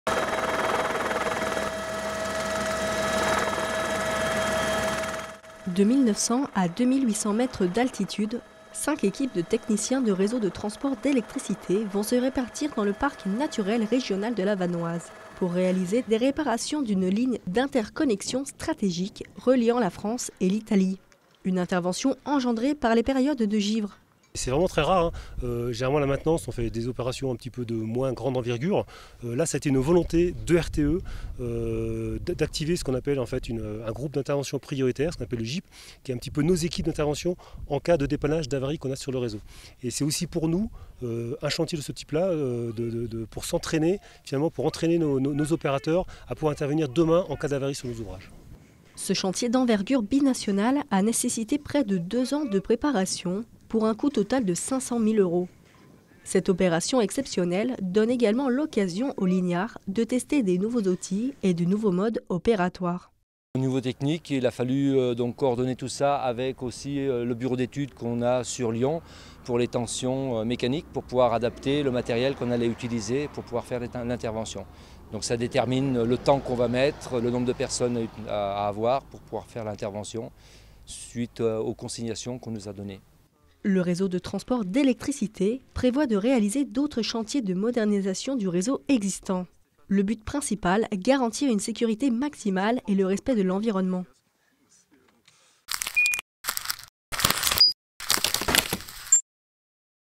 Des travaux dans les airs
C’est une intervention de maintenance hors du commun que réalise actuellement le Réseau de Transport d’Électricité. Du 1er au 12 octobre, 40 spécialistes des lignes électriques venus de toute la France travailler sur la réparation et la maintenance d’une ligne d’interconnexion reliant la France et l’Italie. Plus d’explication avec Kimberley Triviaud